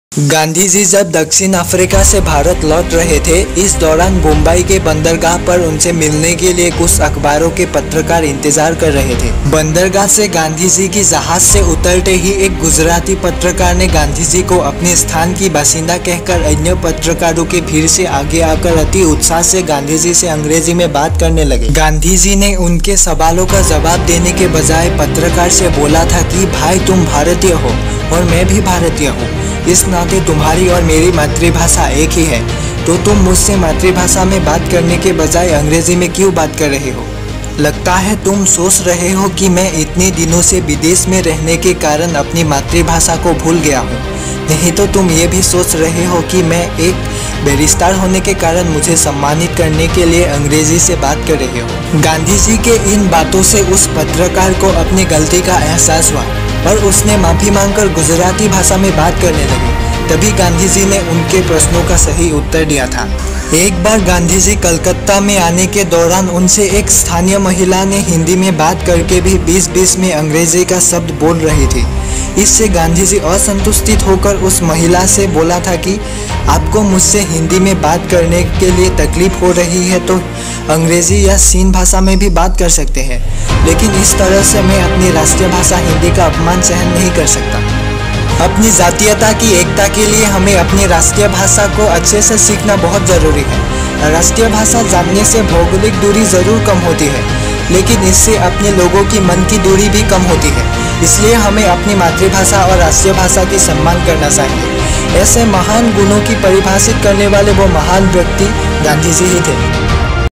राष्ट्रभाषा का सम्मान। राष्ट्रीय पिता। महात्मा गांधी।
यहां राष्ट्रीय पिता महात्मा गांधी का  अपने राष्ट्रीय भाषा के प्रति निष्ठा के बारे में बताया गया है।
# Mahatma Gandhi
# U Official